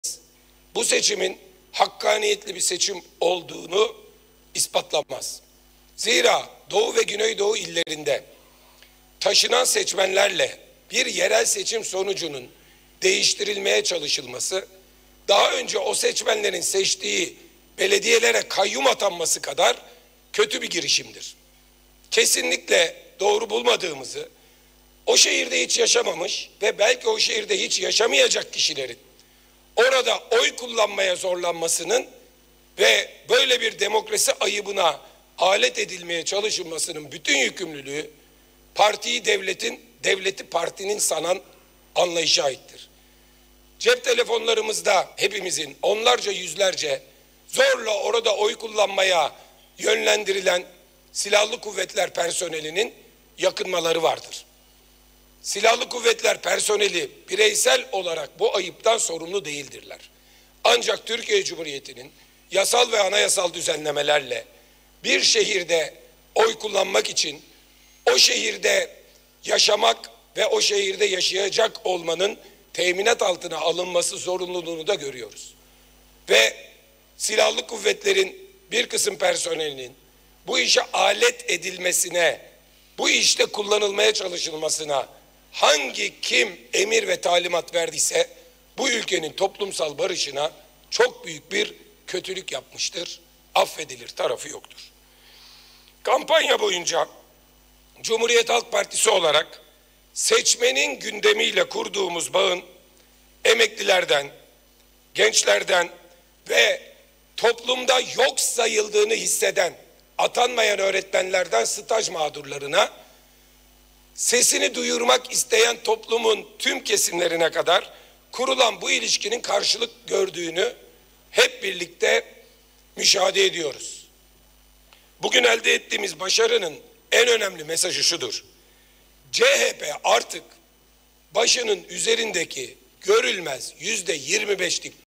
Askerin Görev Yerinde Oy Kullanmasına Tepki Gösterdi! | Özgür Özel | Yerel Seçim
31 Mart 2024 Yerel Seçimleri hakkında CHP Genel Başkanı Özgür Özel açıklama yapıyor.